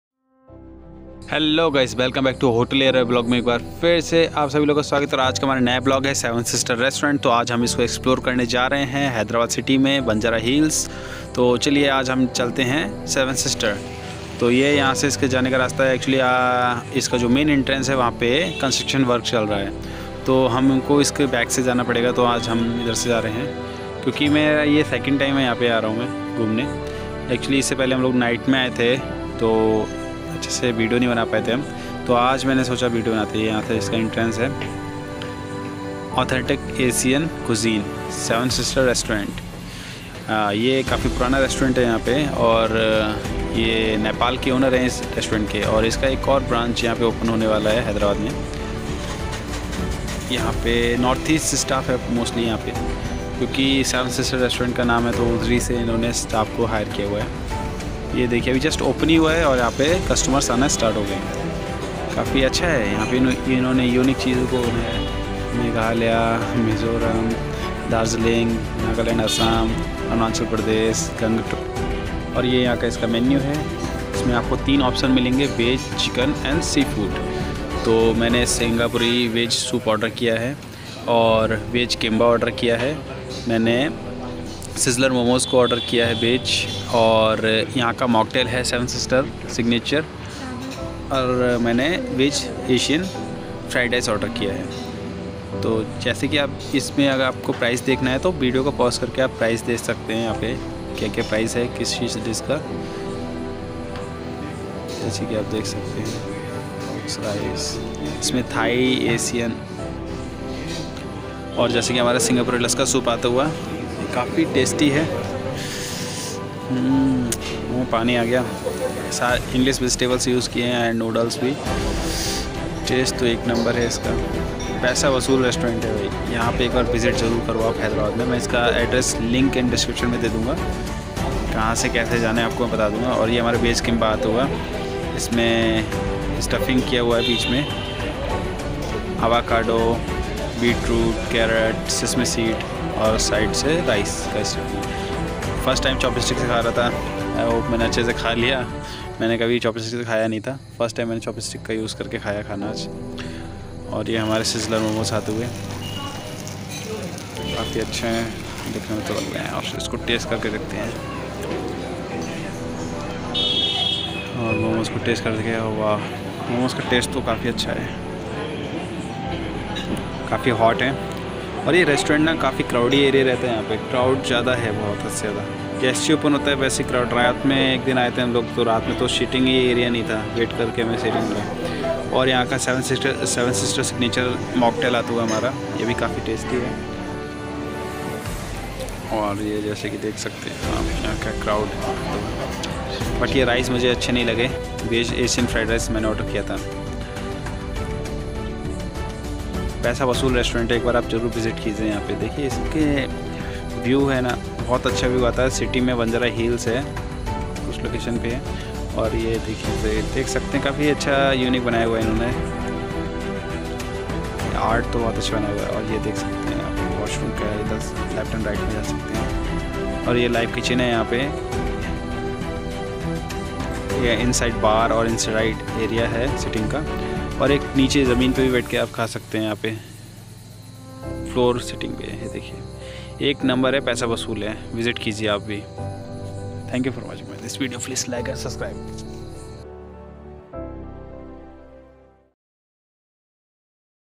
7 Sister Restaurant Asian Authentic Cuisine Best Restaurant in Hyderabad
banjarahills 7sisters youtube india 7 Sister Restaurant Asian Authentic Cuisine Best Restaurant in Hyderabad

Hyderabad: If you want to try authentic north-eastern dishes like Jhol Momos – a special dish where spicy, tangy peanut-based sauce is poured on freshly made momos – or Gurkha Thali, a wholesome Sikkim dish, then head to 7sisters.

Head chef Kismat Linbu describes Khao Swe which is a Burmese dish that comes with fried onions, chilies, peanuts, spices, boiled noodles, coconut milk broth, bean fritters, and lime as his favorite dish. "While the dishes' roots are Burmese, it is a popular dish in all the north-eastern states. This particular dish is served in bamboo baskets which I brought from my hometown in Assam. It's how we eat and we want people in Hyderabad to have the authentic experience while eating," he says.

Jhol Momos
Kismat sayd Jhol Momos is one of the dishes always in demand at 7sisters. One of the main ingredients in this dish is Jhol Achat, Nepali hog plum.

"We make some dishes slightly spicier to cater to the Hyderabadi populations' taste buds," Kismat says.

7 sisters Banjara Hills is the new outlet. There is another in Hitex, Kondapur.